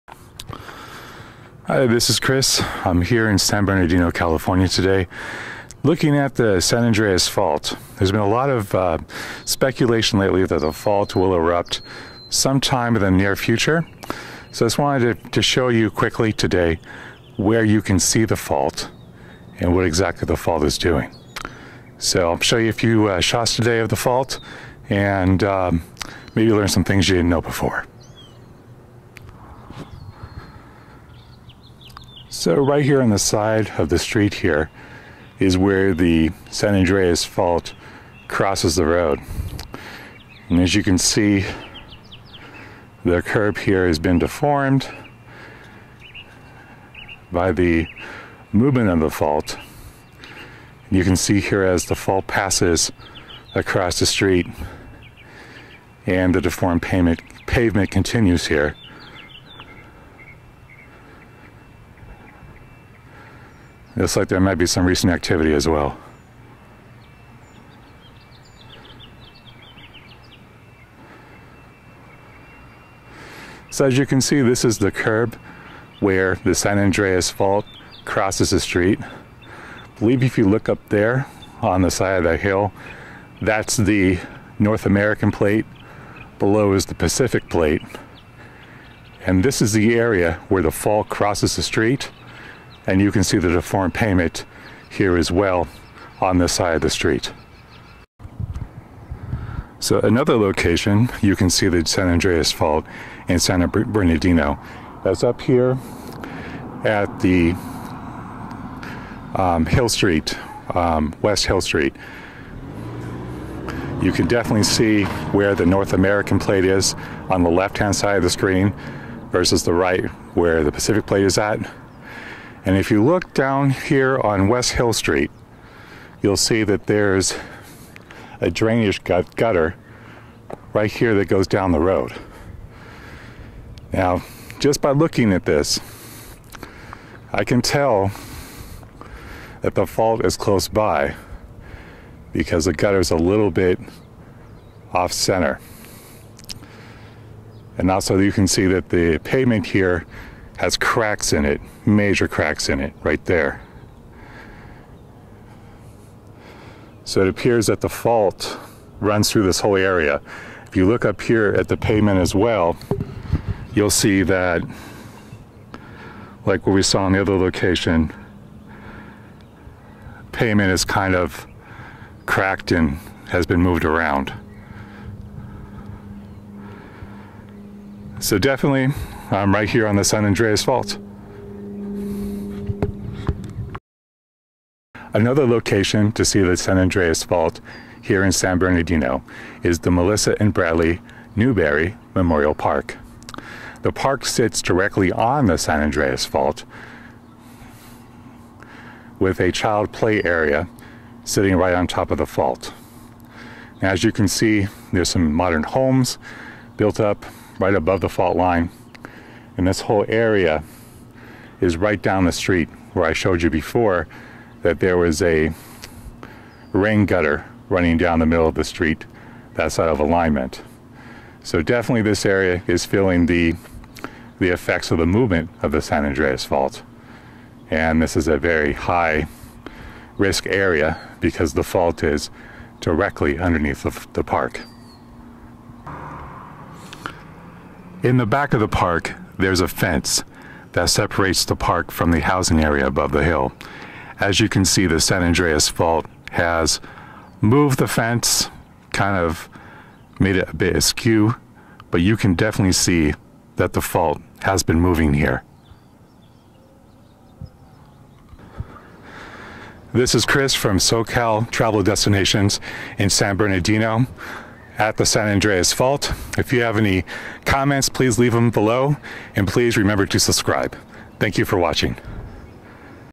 Tour of the San Andreas Fault in San Bernadino
A brief tour of the San Andreas Fault as it runs through a hillside community in San Bernadino, CA. If you are in the area, check out Newberry park at 560 W Hill Dr, San Bernardino, CA 92407. The fault runs along the back fence of the park.